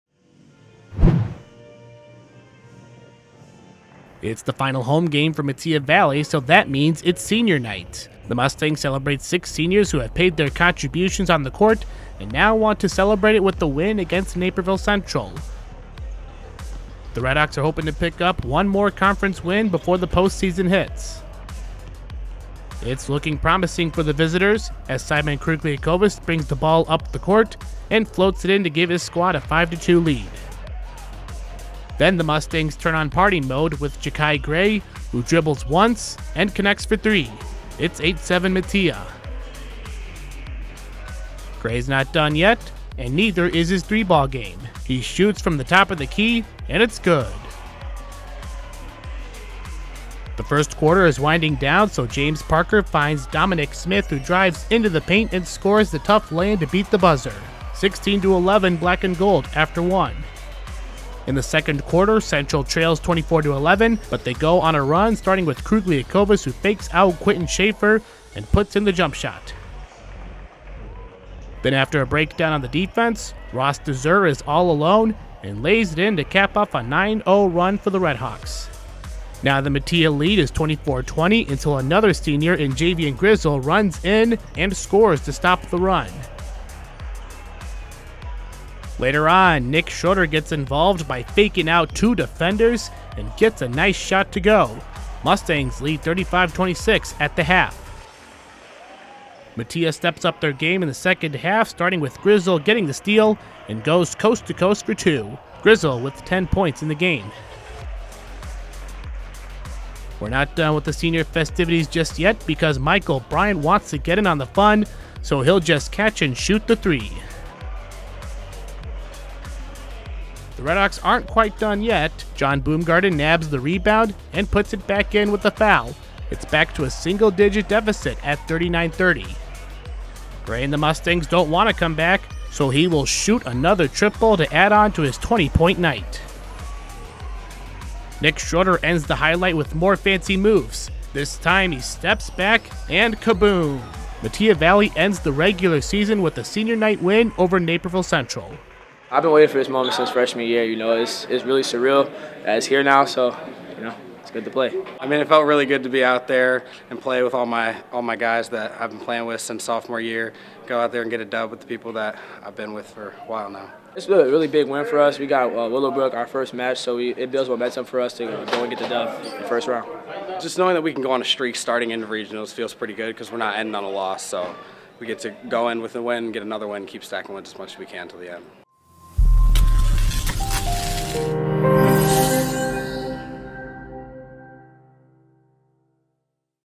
Metea Valley boys basketball celebrates senior night with win over Naperville Central
It’s the final home game for Metea Valley so that means it’s senior night. The Mustangs celebrate six seniors who have paid their contributions on the court and now want to celebrate it with a win against Naperville Central. The Redhawks are hoping to pick up one more win before the postseason hits.

It’s looking promising for the visitors. Simon Krugliakovas brings the ball up the court and floats it in to give his squad a 5-2 lead.

Jahki Gray turns it on from three

Then the Mustangs turn on party mode with Jahki Gray who dribbles once and connects for three. It’s 8-7 Metea.

Gray’s not done yet and neither is his three ball game. He shoots from the top of the key and it’s good.

The first quarter is winding down so James Parker finds Dominic Smith who drives into the paint and scores the tough lay in to beat the buzzer. 16-11 black and gold after one.

Redhawks go on a run

In the second quarter Central trails 24-11 but they go on a run starting with Krugliakovas who fakes out Quentin Shaffer and puts in the jump shot.

Then after a breakdown on the defense, Ross Dezur is all alone and lays it in to cap off a 9-0 run for the Redhawks.

Now the Metea lead is 24-20 until another senior in Javion Grizzle runs in and scores to stop the run.

Later on Nick Schroeder gets involved by faking out two defenders and gets a nice shot to go. Mustangs lead 35-26 at the half.

Mustangs run away in the second half

Metea steps up their game in the second half starting with Grizzle getting the steal and goes coast to coast for two. Grizzle with ten points in the game.

We’re not done with the senior festivities just yet because Michael Bryant wants to get in on the fun so he’ll just catch and shoot the three.

The Redhawks aren’t quite done yet. John Boomgarden knabs the rebound and puts it back in with a foul. It’s back to a single digit deficit at 39-30.

Gray and the Mustangs don’t want a comeback so he will shoot another triple to add on to his twenty point night.

Nick Schroeder ends the highlight with more fancy moves. This time he steps back and he got it. Metea Valley ends the regular season with a big win over Naperville Central.